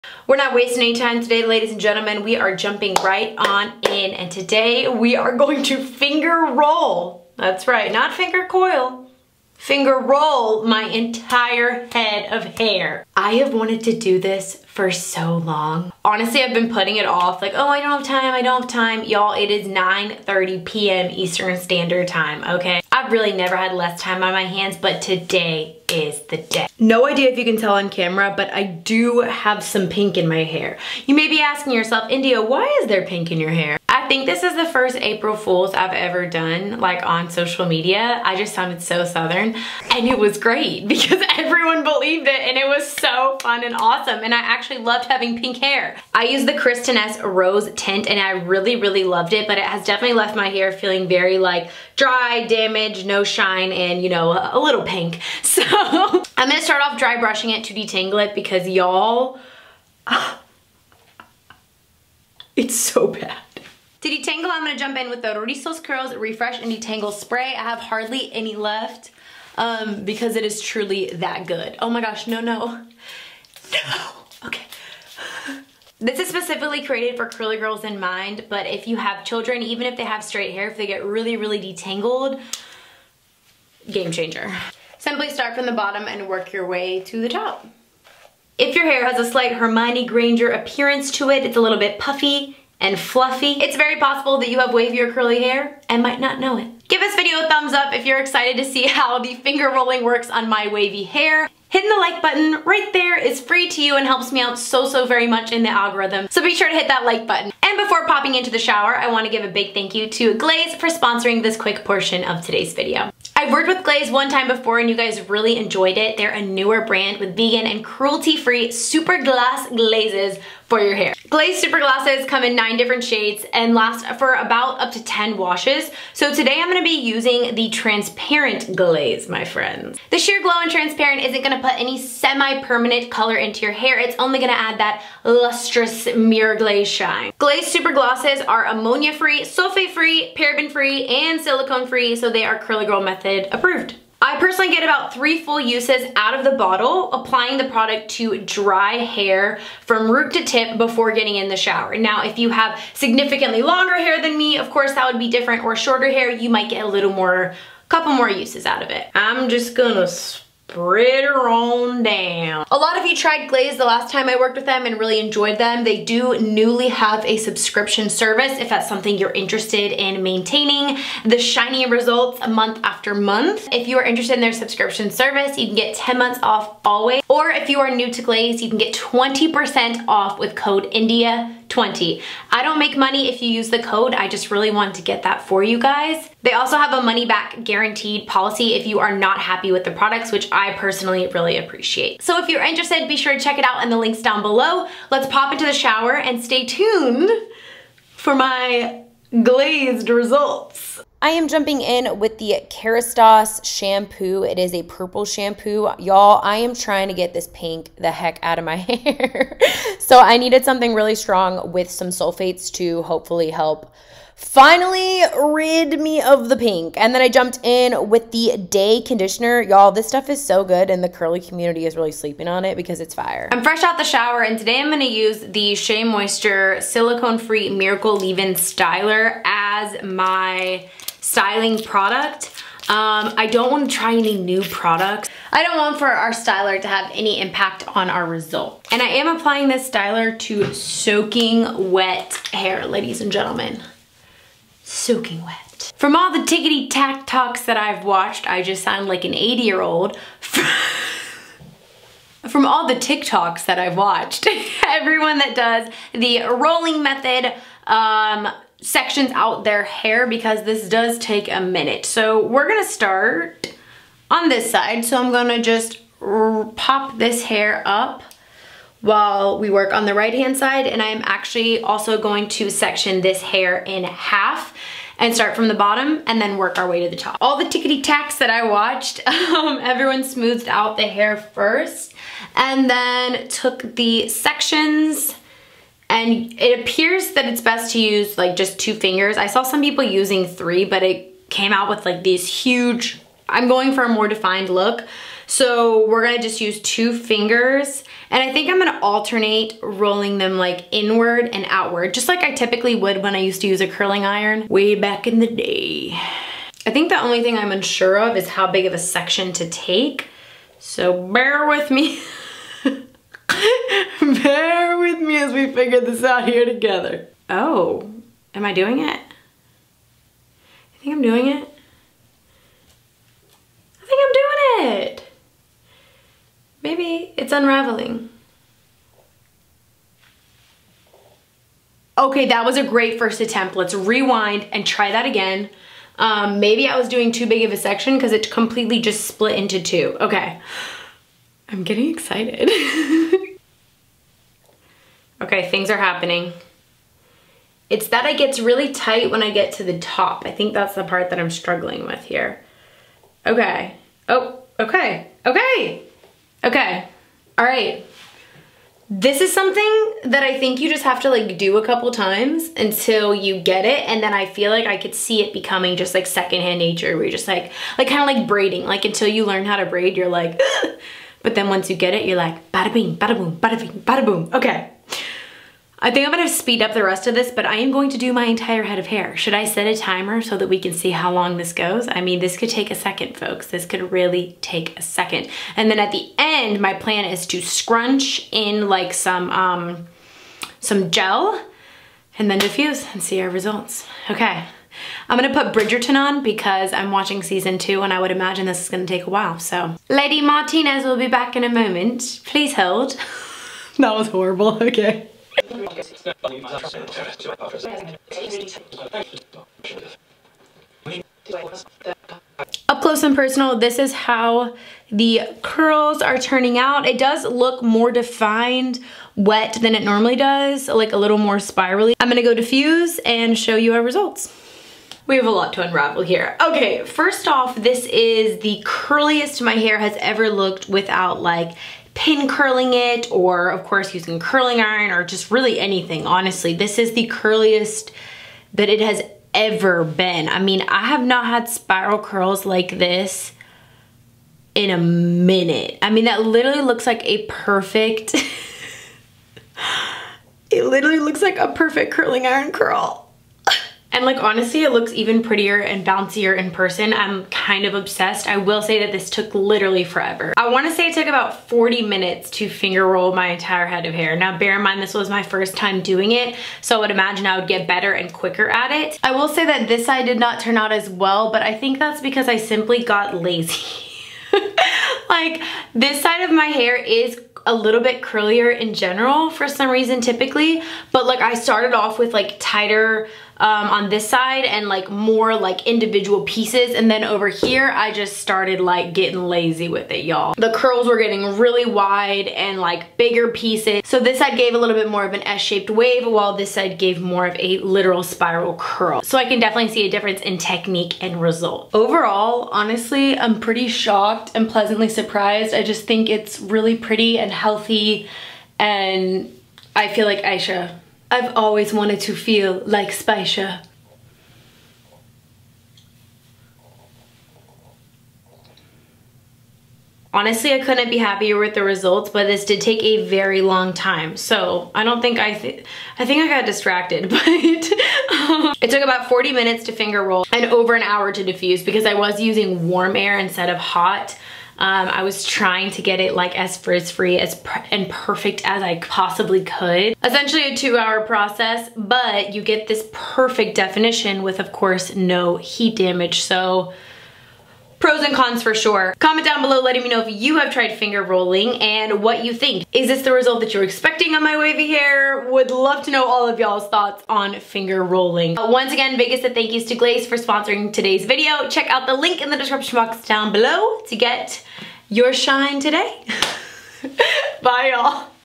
FINGER ROLLING WAVY HAIR! TRAIN YOUR HAIR TO BE CURLIER 😍
pink hair spray( ON SALE)
detangle spray
purple shampoo